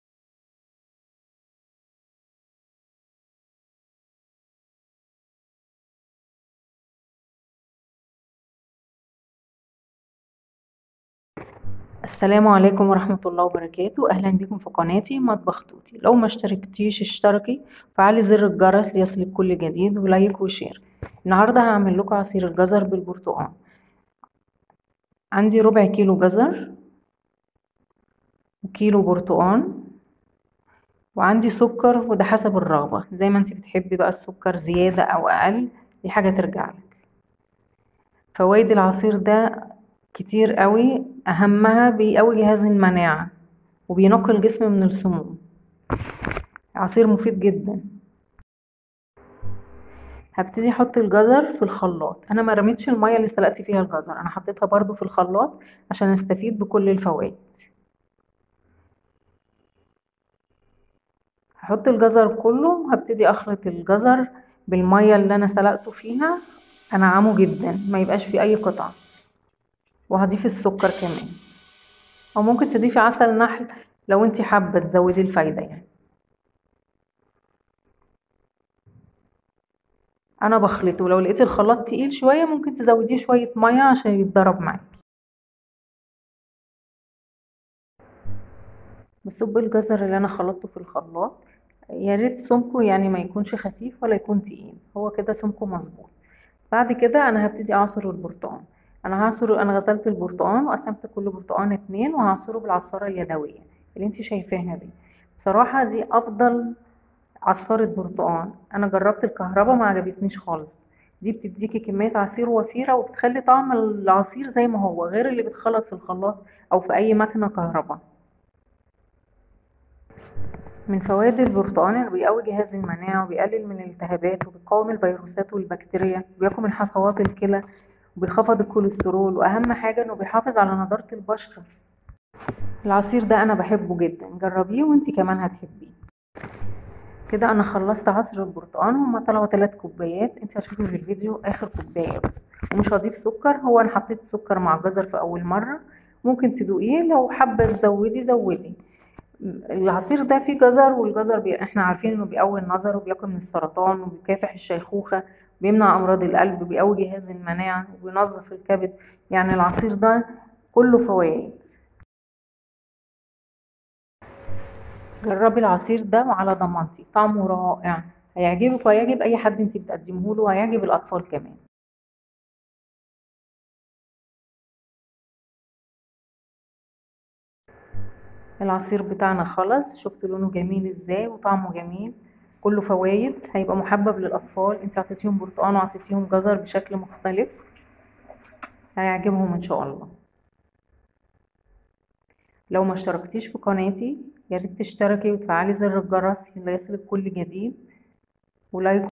عصير مقاوم للفيرو وسات🏵🏵👌 و طعمة حلو جدا و كلة فوايد مطبخ_توتى
عصيرمقاوم للفيروسات🏵🏵👌 و طعمة حلو جدا وكلة فوايد مطبخ توتى
المقادير

برتقال
جزر سكر
مشروب لبرتقال الصحى
عصير البرتقال الفرش
مربى البرتقال